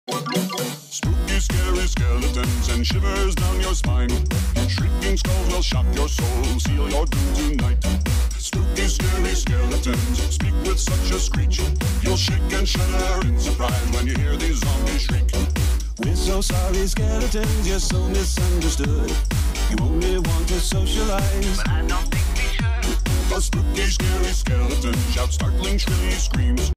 Manucure halloween avec plaque stamping | Passione Beauty
Avec les Painting Gels et la plaque stamping Monsters, réalise cette super manucure Halloween!